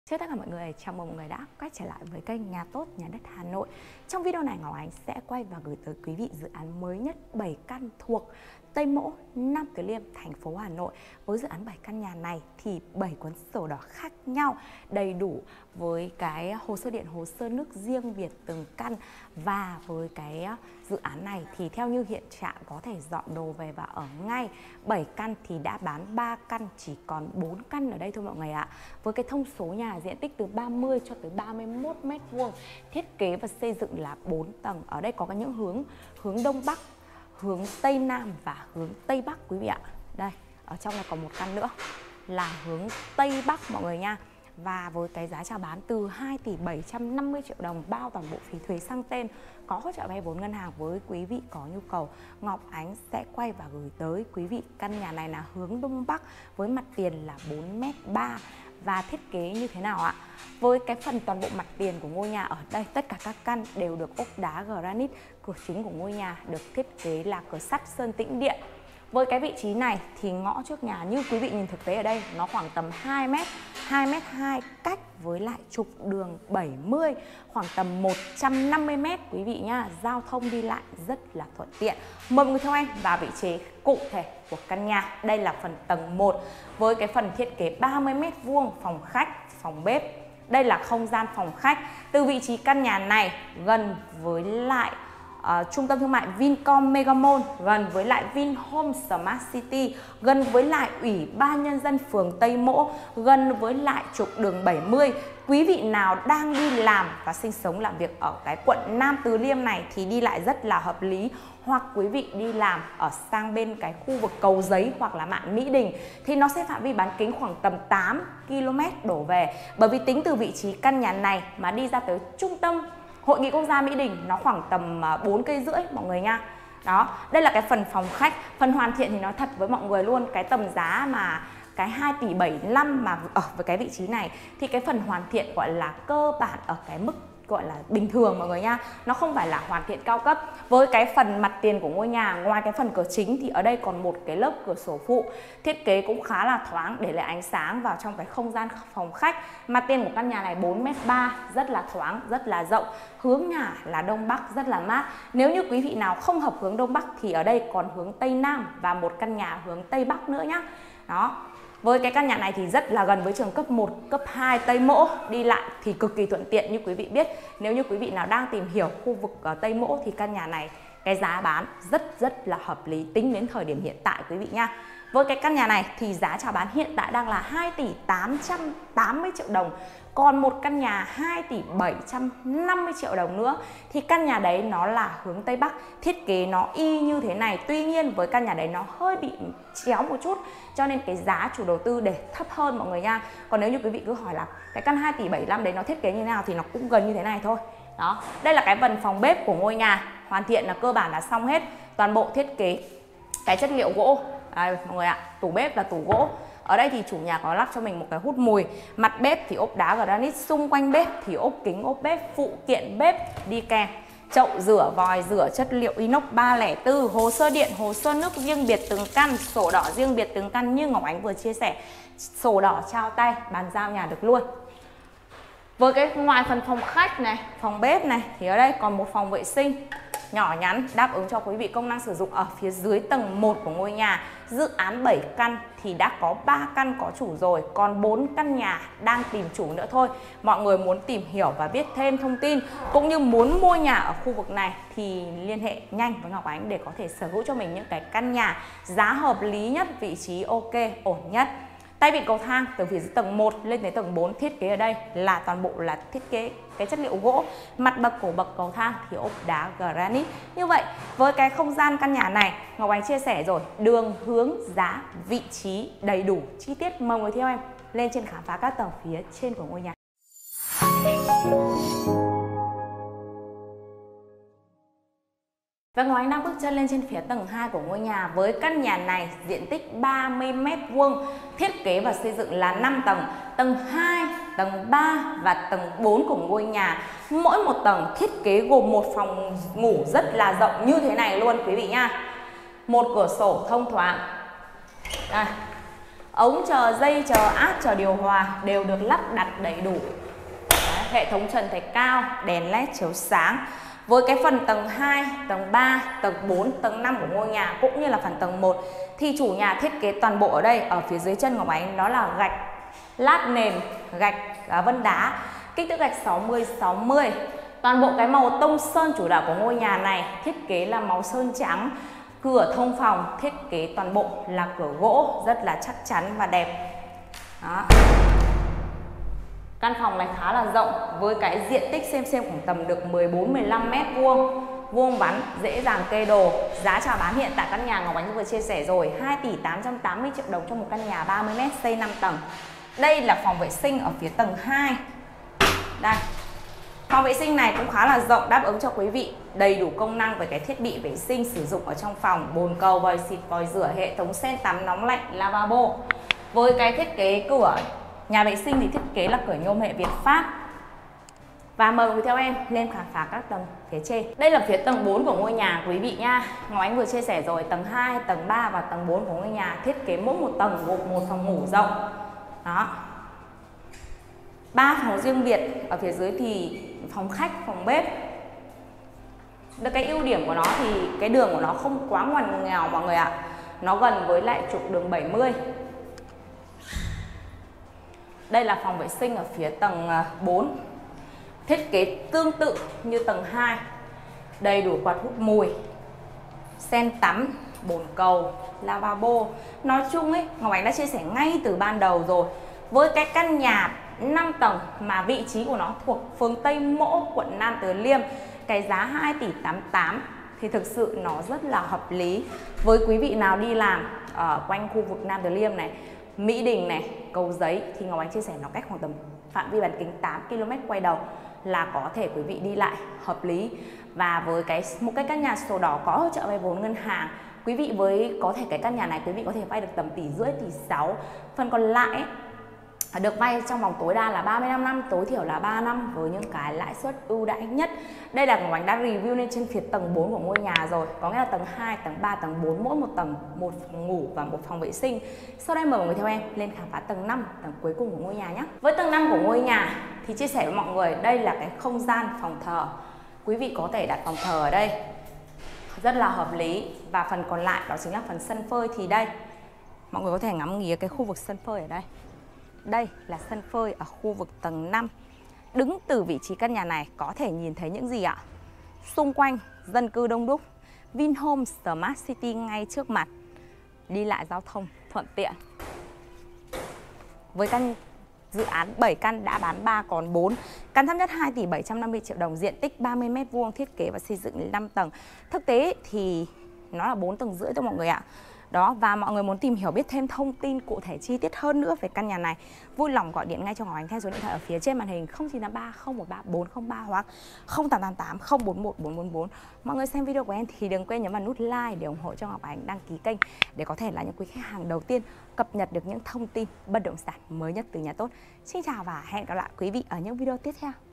GẦN VINCOM MEGA MALL TÂY MỖ 5 tầng - 3 phòng ngủ, trả trước từ 1 tỷ 200 triệu | nhà tốt
- Địa chỉ: đường Tây Mỗ, Nam Từ Liêm, Hà Nội
- Diện tích: 30m2
- Hướng nhà: Đông Bắc
- Thiết kế: 5 tầng, 3PN, phòng khách, bếp, phòng thờ, sân phơi, 4wc
- Giá bán: 2,88 tỷ